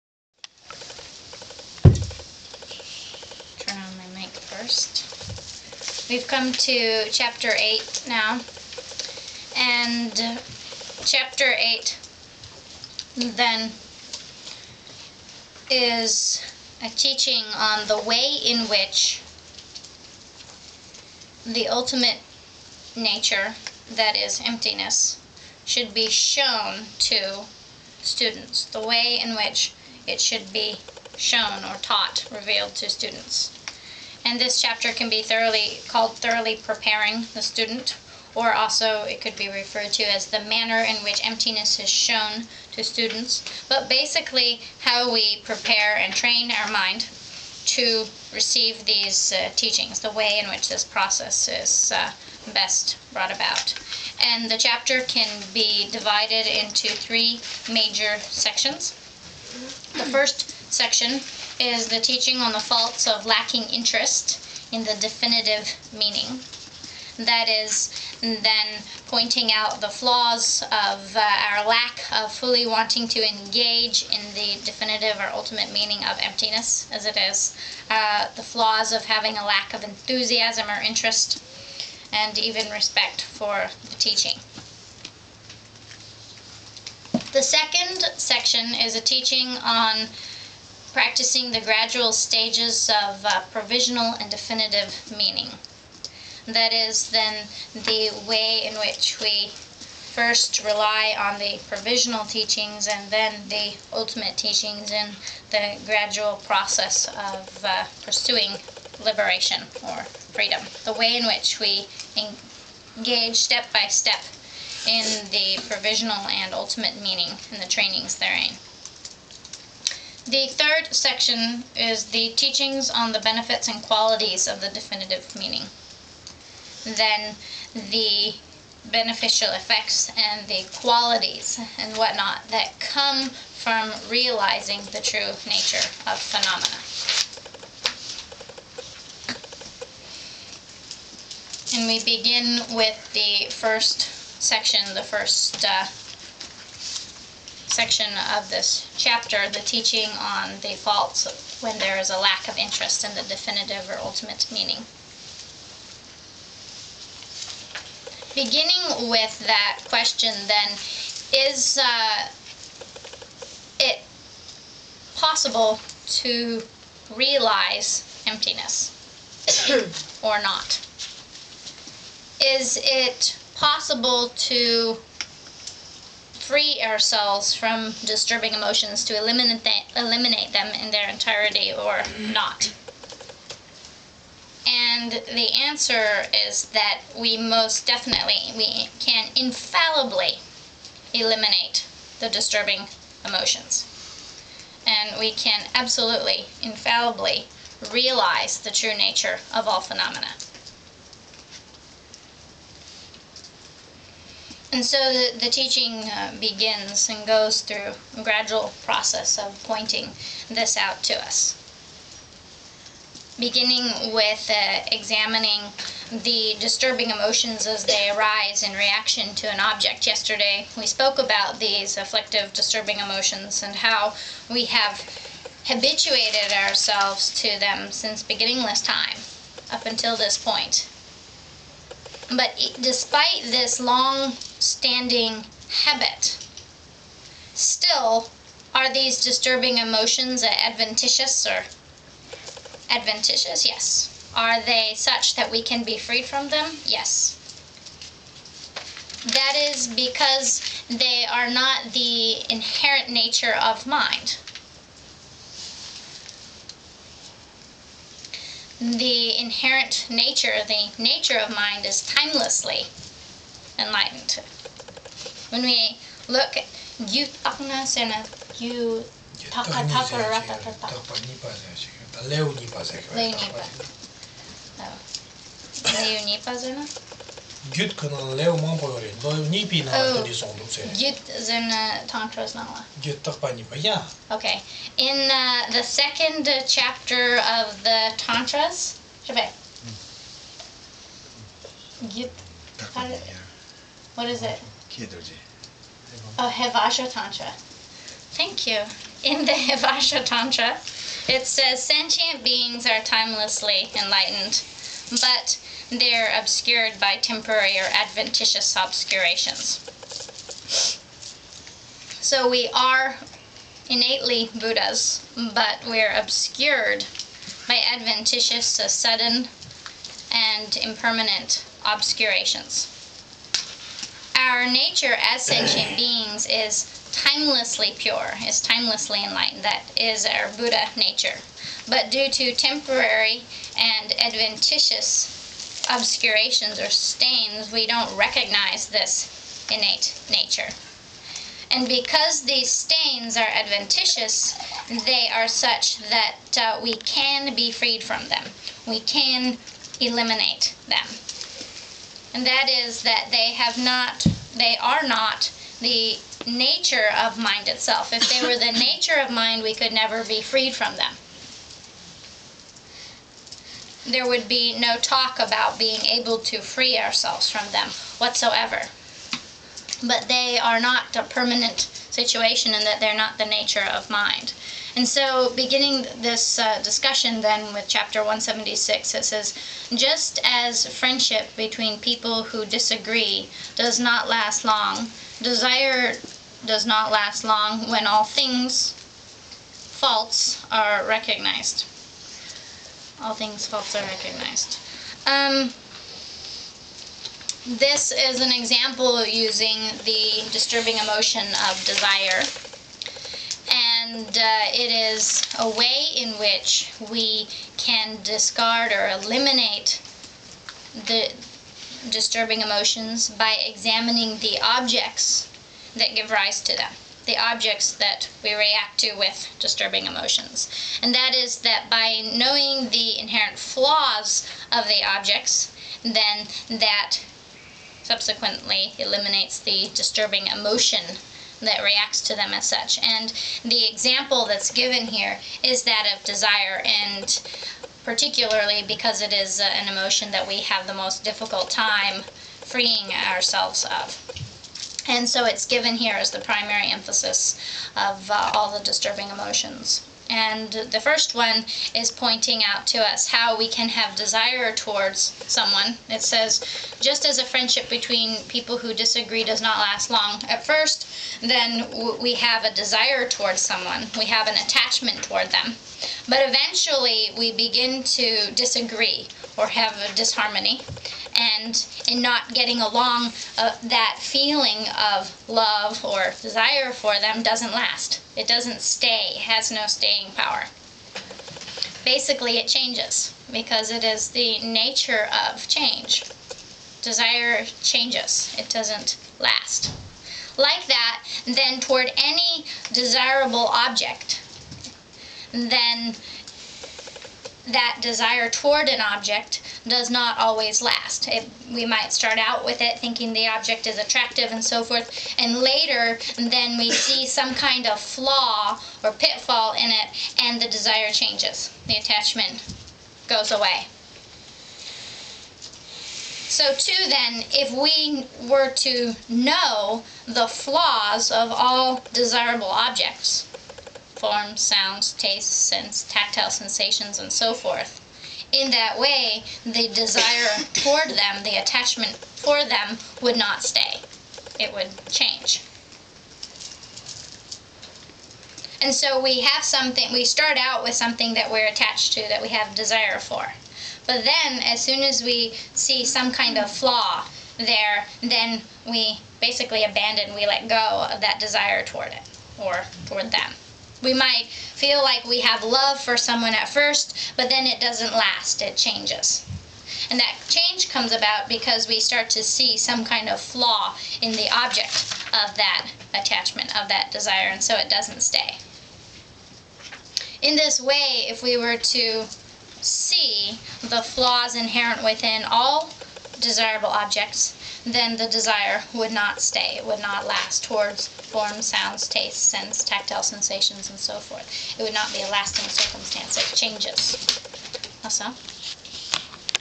Khentrul Lodro Thaye Rinpoche 400 Verses Part 2
Khentrul Lodro Thaye Rinpoche
Tibetan teaching excerpt of the 400 Verses of Aryadeva
Translated to English by Paloma Lopez Landry